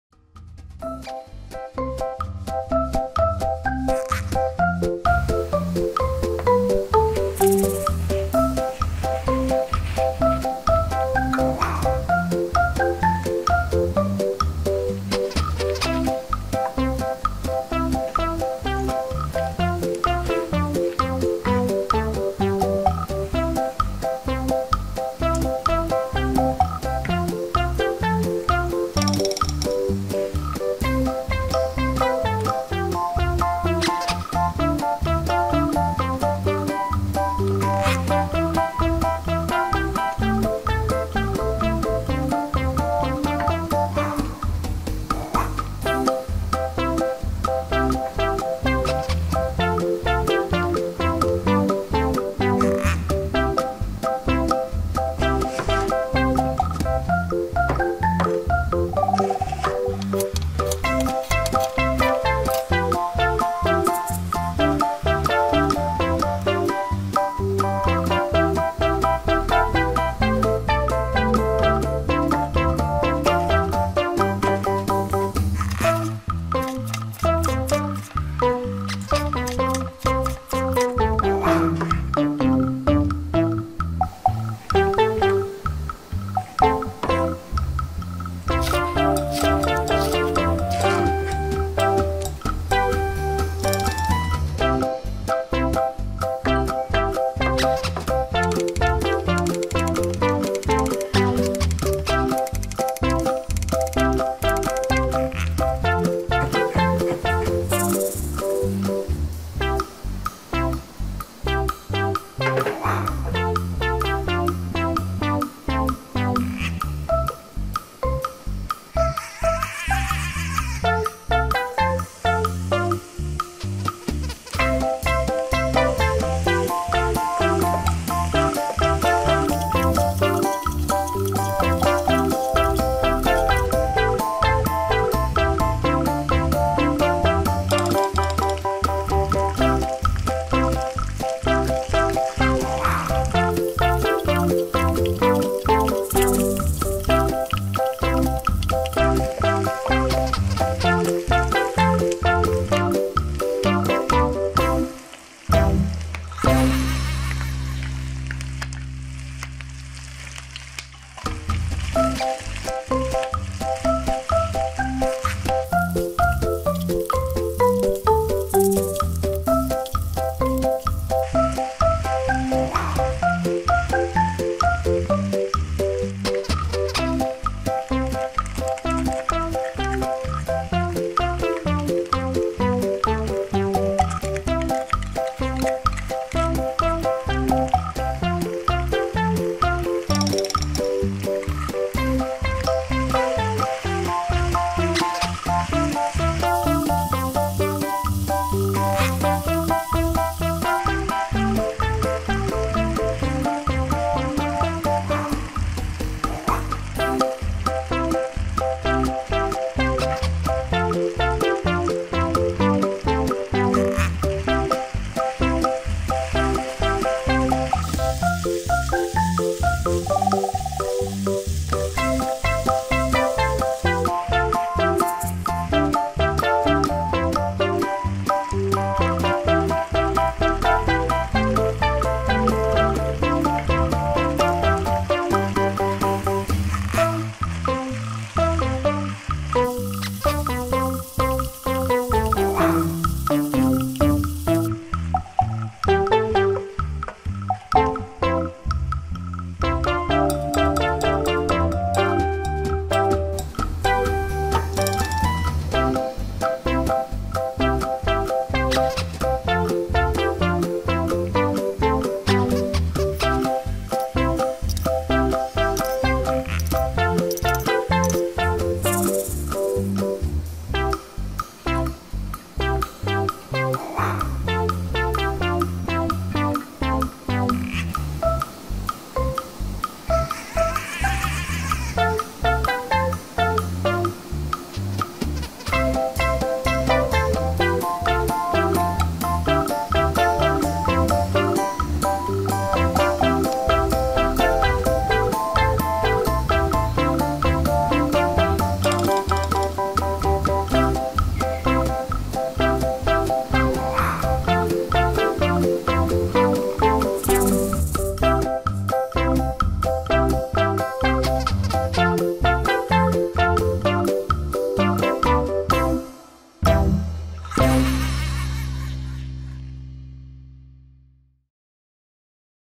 วิธีทำ หมูผัดพริกเผา สอนทำกับข้าวง่ายๆ มือใหม่ก็ทำได้ไม่ยาก I stir pork with chilli paste I
พริกเผา 1 ช้อนโต๊ะ